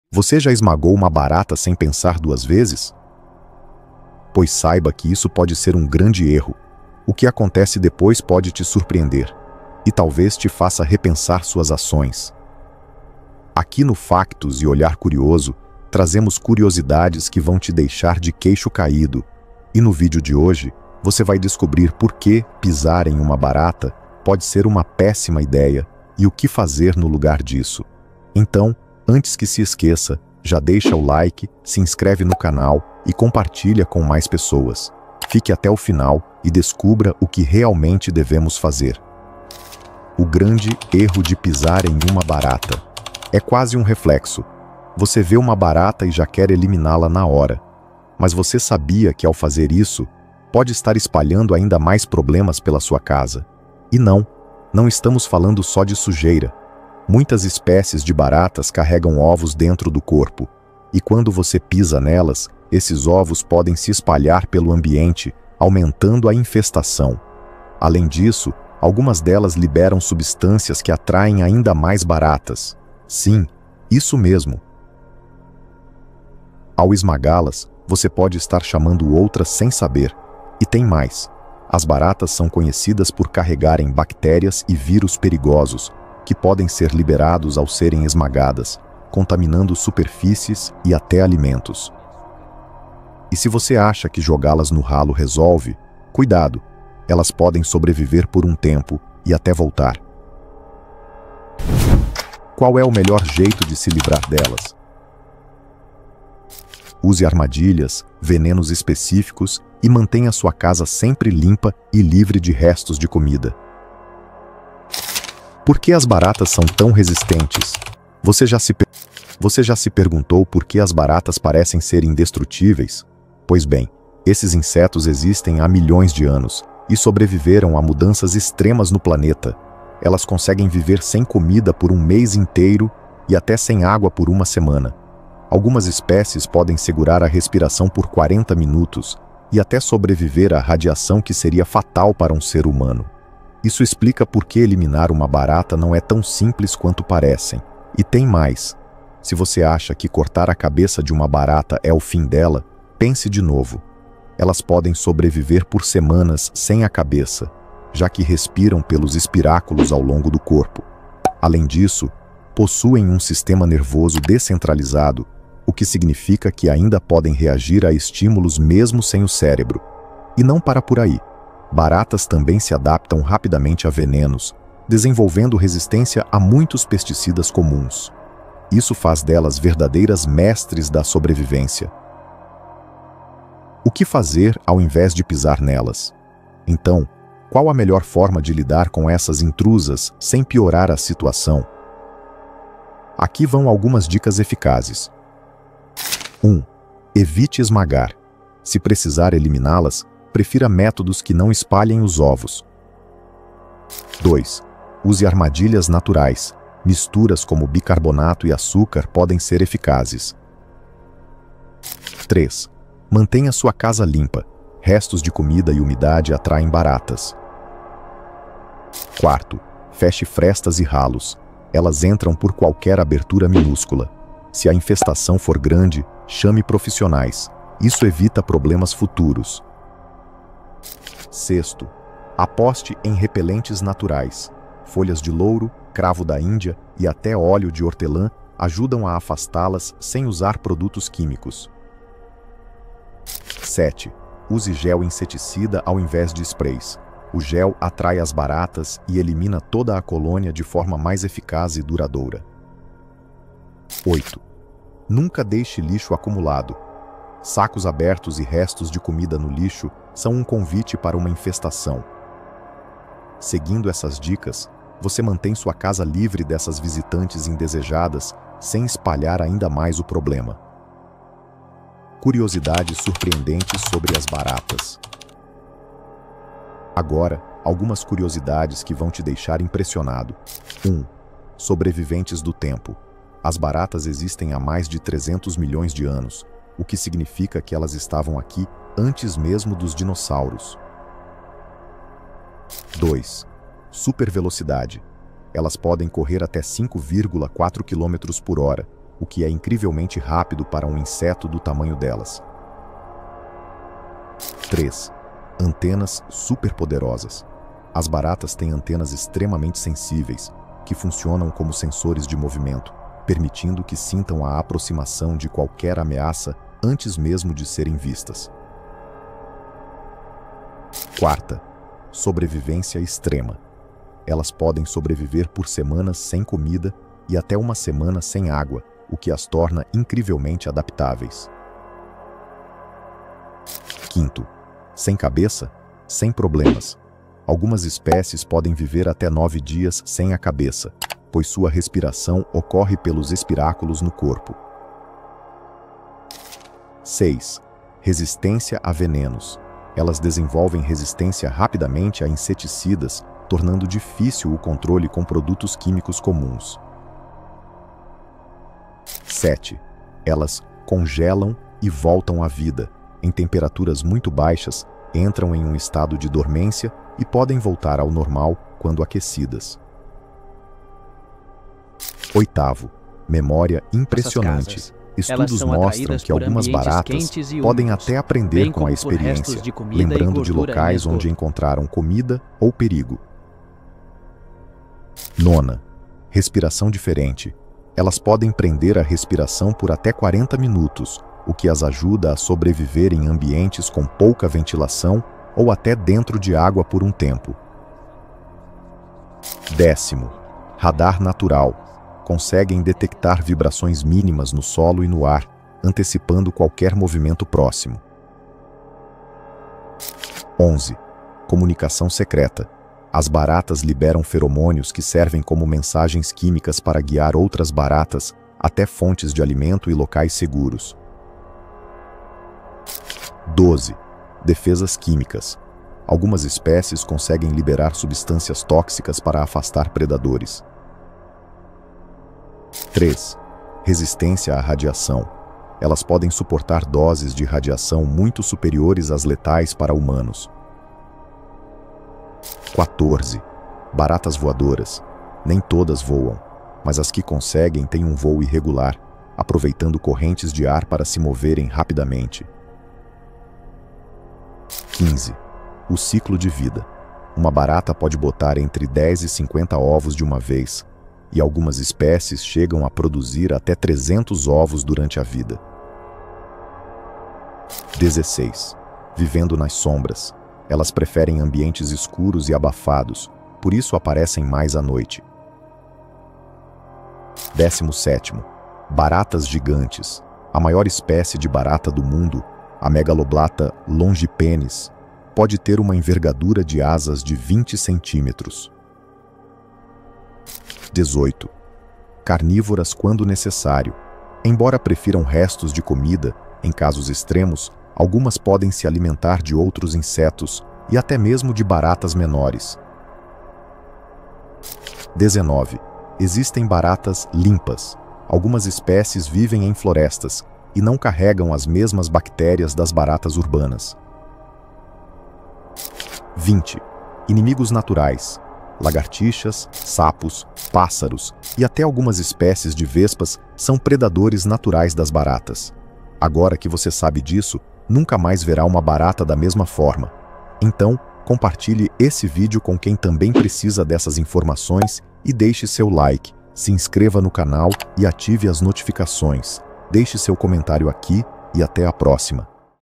Nunca Mais Pise em uma Barata Antes de Ver Isso! O Que Acontece Vai te Surpreender!
Nunca Mais Pise em uma Barata Antes de Ver Isso! O Que Acontece Vai te Surpreender!Você já esmagou uma barata sem pensar duas vezes? ⚠️
Isso pode ser um grande erro, e o motivo vai te surpreender! O que acontece depois pode piorar a infestação na sua casa sem que você perceba.

Neste vídeo, você vai descobrir por que NUNCA deve pisar em uma barata, o que fazer no lugar disso e fatos incríveis sobre esses insetos que sobreviveram por milhões de anos!

As baratas são extremamente resistentes: podem viver sem comida por semanas, sobreviver sem cabeça e até congelar e "reviver" depois! 😱

➡️ Fique até o final para aprender o jeito certo de se livrar delas sem espalhar ovos ou atrair mais baratas para sua casa!

🔔 Ative as notificações para não perder nenhum fato surpreendente!